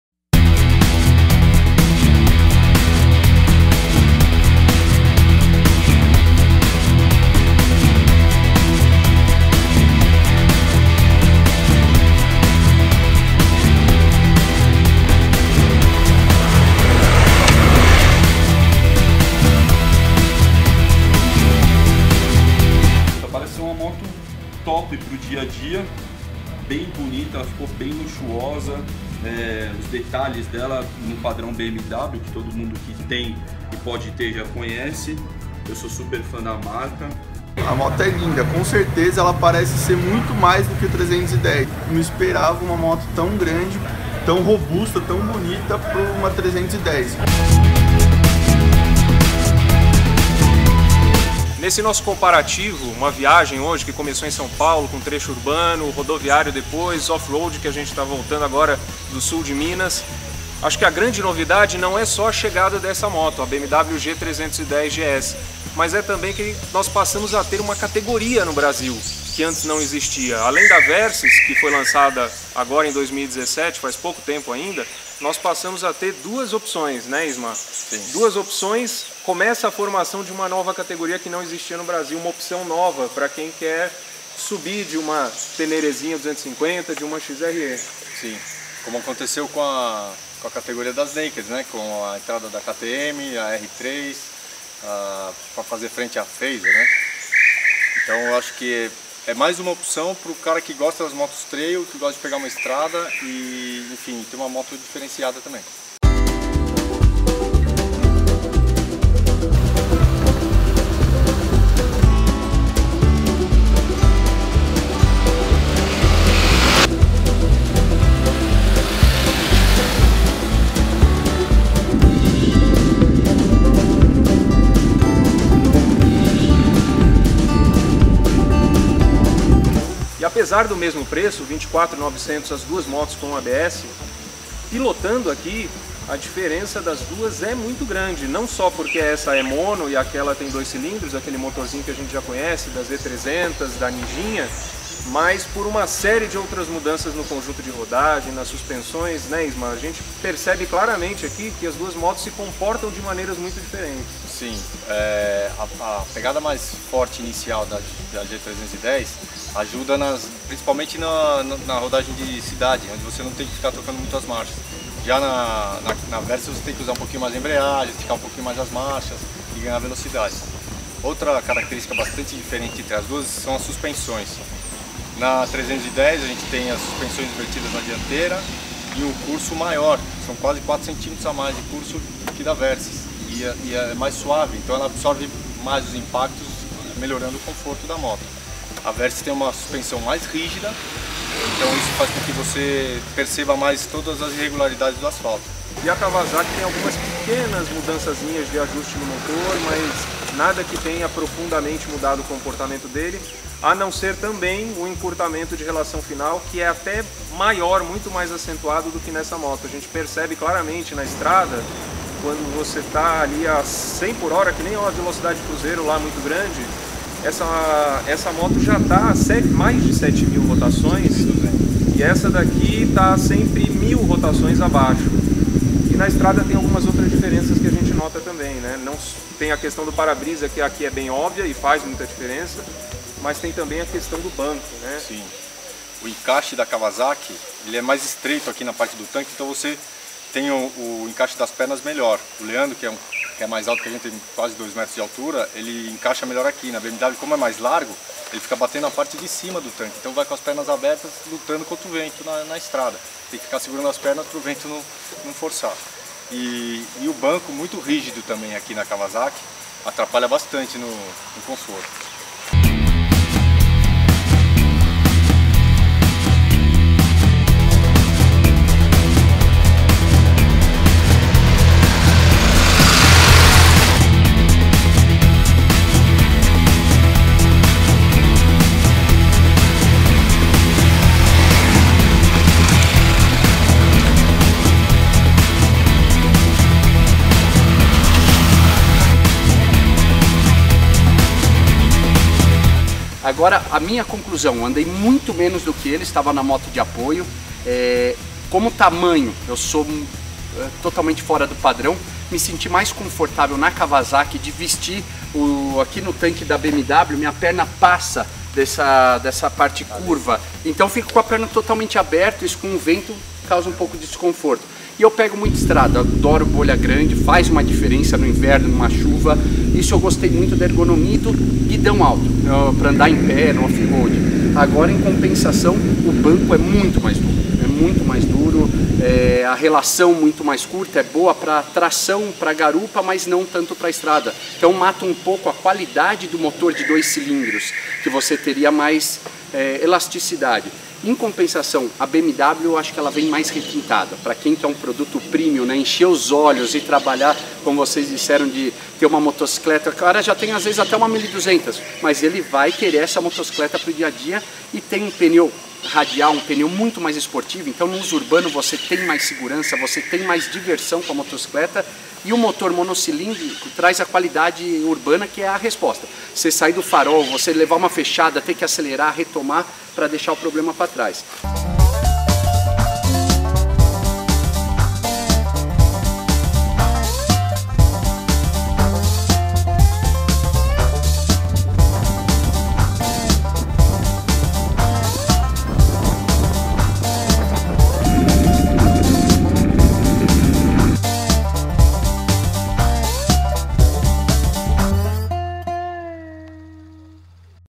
COMPARATIVO: NOVA BMW G310GS x KAWASAKI VERSYS-X
A BMW G310GS acaba de ser lançada no Brasil e já testamos com a Kawasaki Versys-X 300!